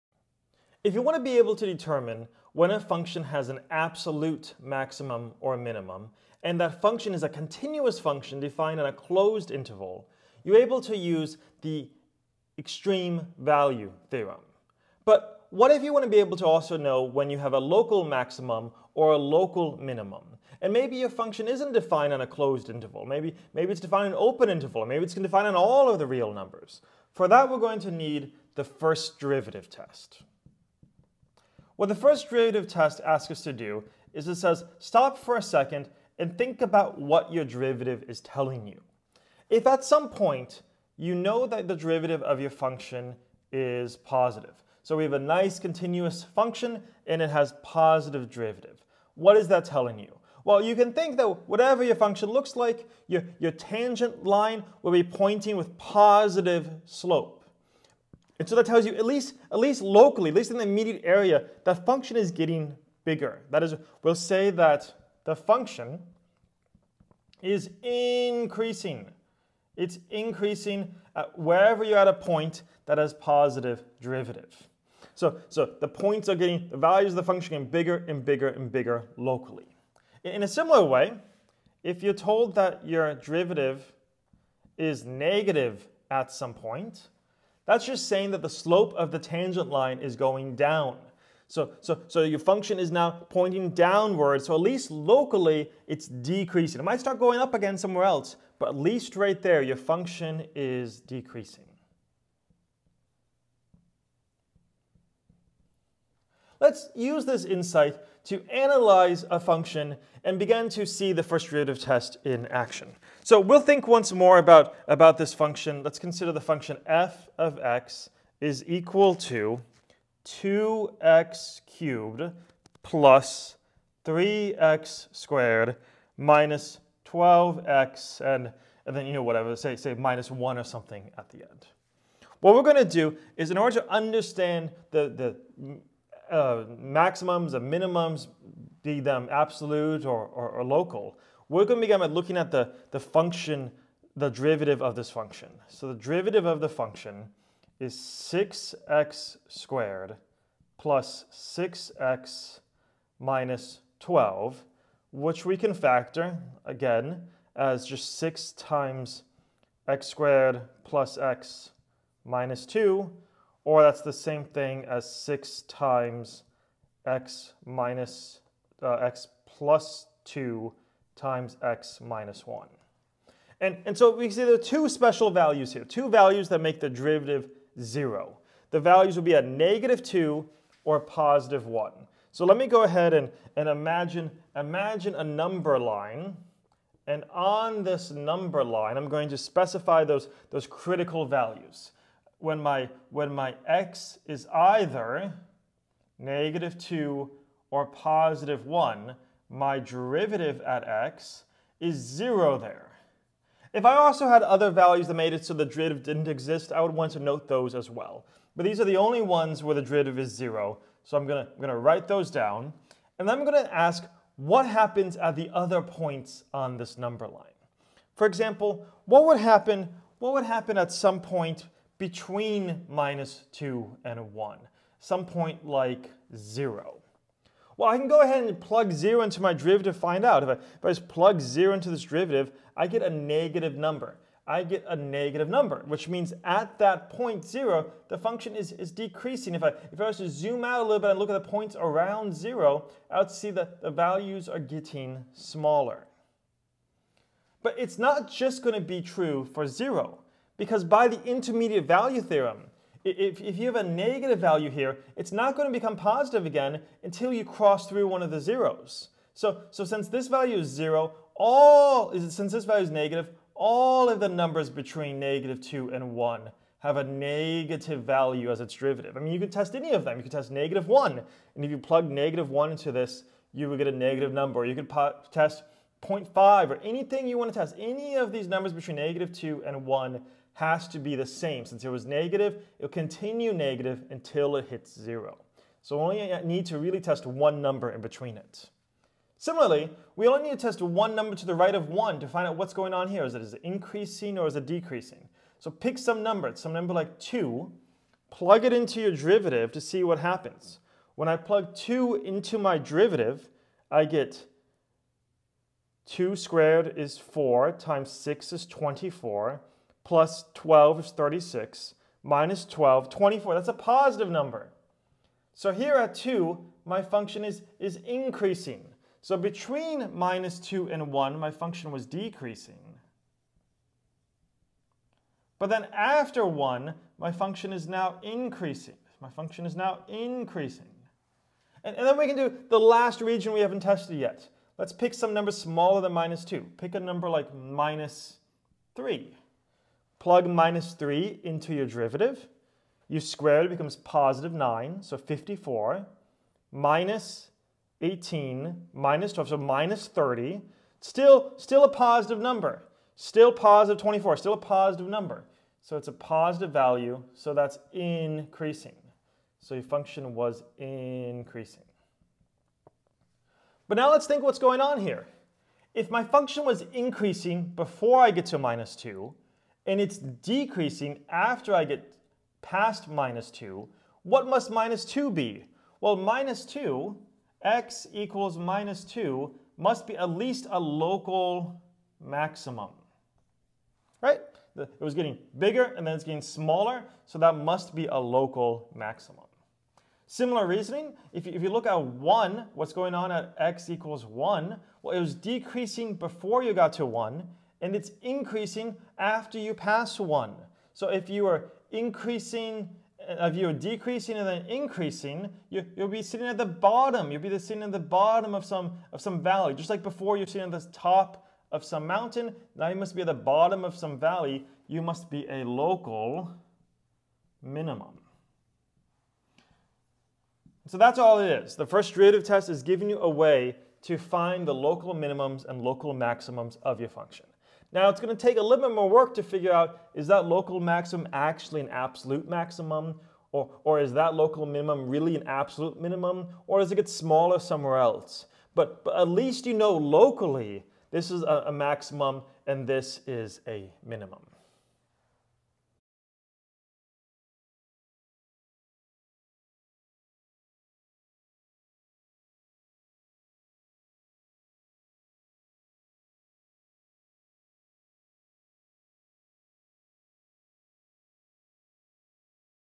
Lit Calculus 43: First Derivative Test
Presented by Anthony Bosman,  PhD.
Math at Andrews University: math.andrews.edu

Introduces the first derivative test to determine local maximum and minimum.